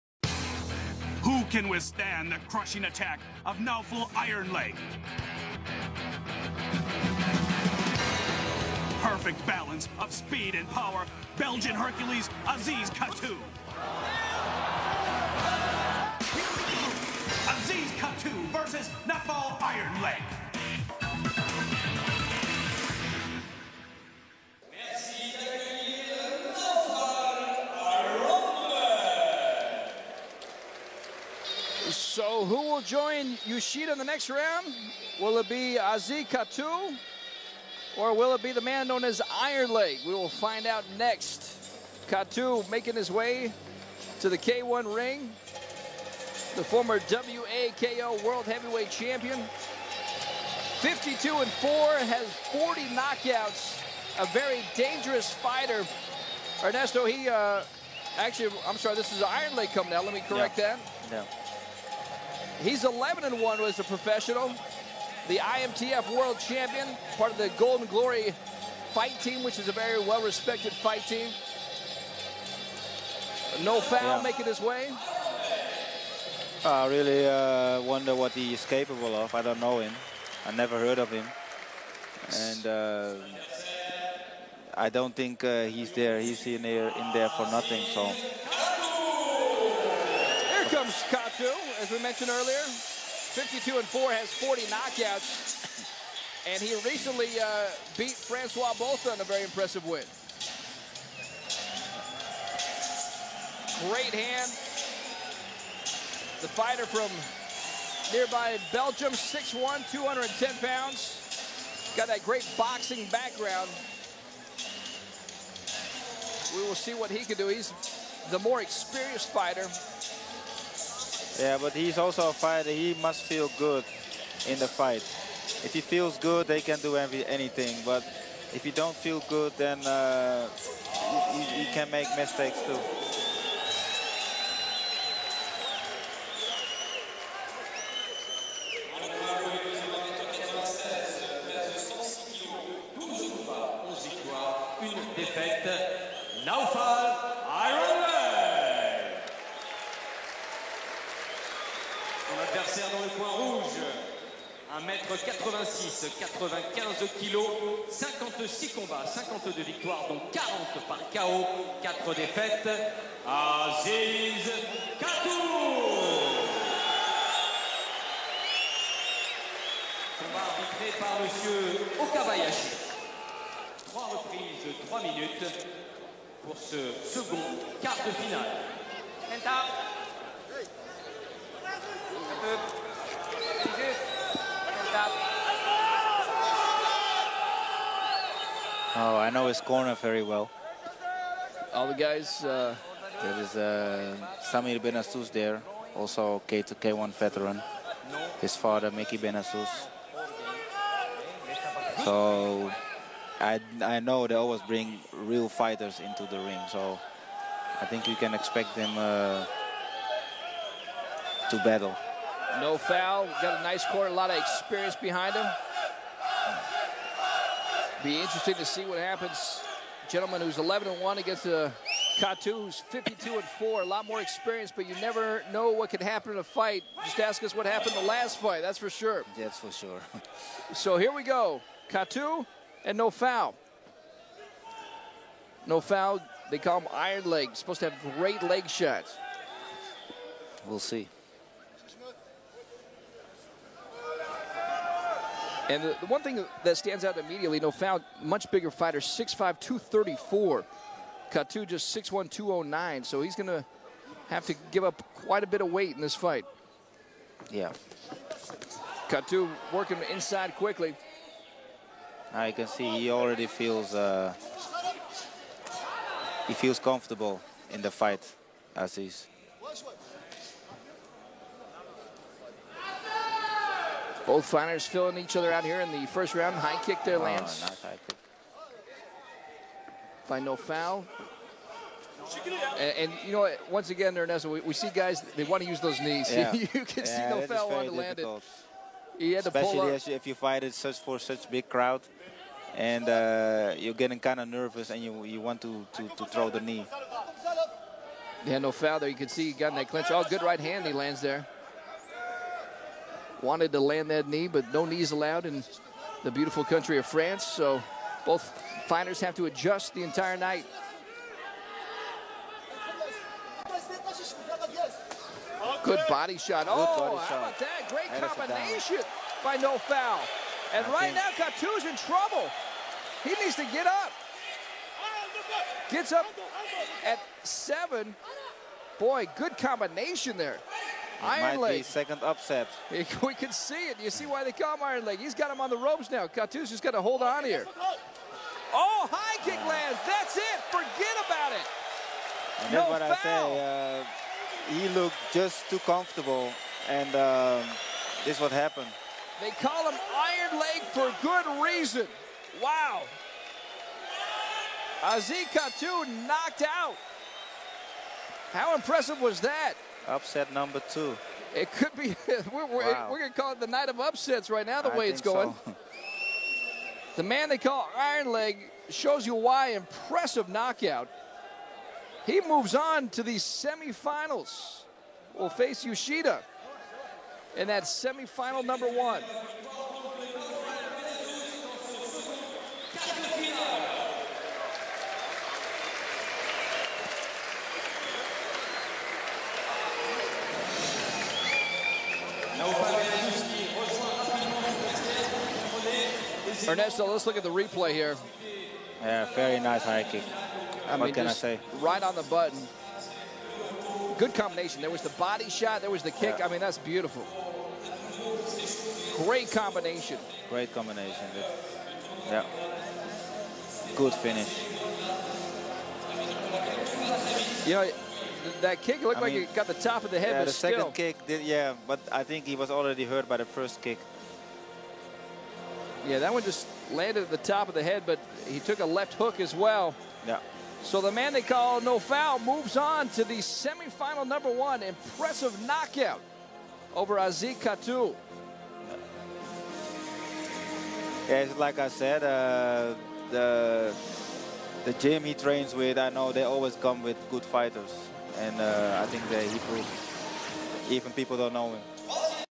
Aziz Khattou Vs. Naoufal Ben Azzouz (27/05/2005) [Alternate Commentary]
Aziz Khattou Vs. Naoufal "Iron Leg" Ben Azzouz
K-1 World Grand Prix 2005 in Paris
May 27, 2005 | Paris, France

Heavyweight bout with K-1 rules, and tournament quarterfinal.